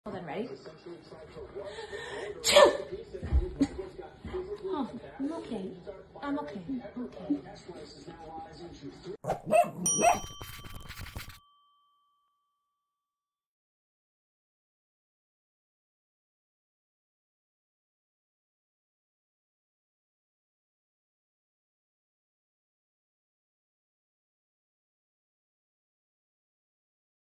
German Shepherd rescues owner from a sneeze
This long haired GSD named Archie saves his owner from a sneeze. Cuteness overload!